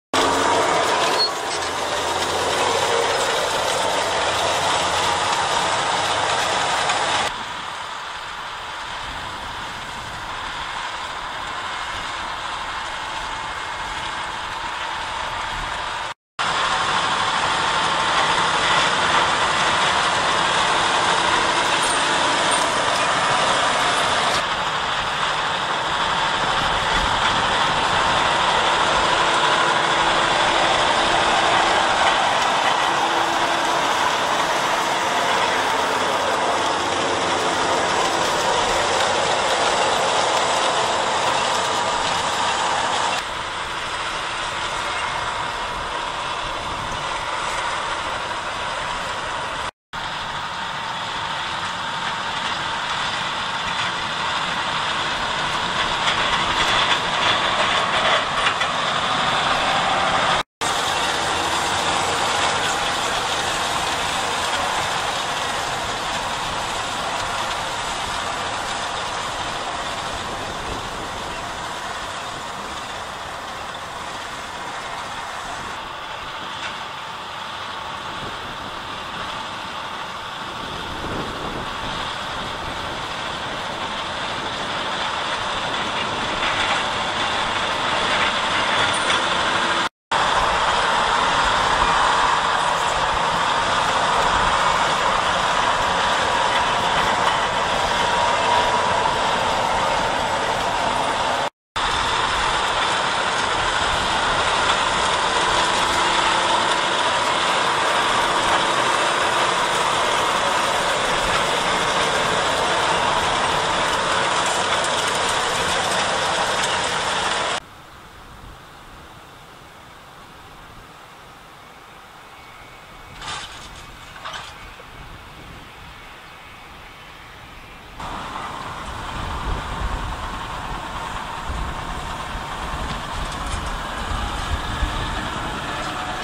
JohnDeere 6155R mit Kverneland.
Maisaussaat in Kleve-Kellen, NRW, Deutschland im Mai 2023.